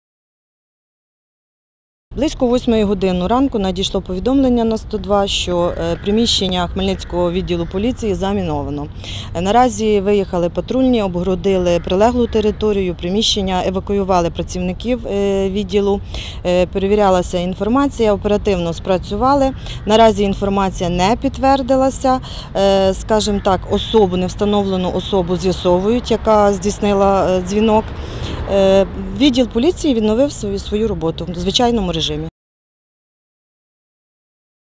У Хмельникцьому шукають "підривника" поліції
У Хмельницькому відділі поліції шукали вибухівку.  Про замінування правоохоронців повідомила невстановлена особа. На місце одразу виїхали патрульні та вибухотехніки.  На час обстеження території тимчасово був перекритий рух автотранспорту по вулиці Пушкіна. Як повідомляють у відділі поліції, факт замінування не пітвердився. Правоохоронці вже шукають жартівника.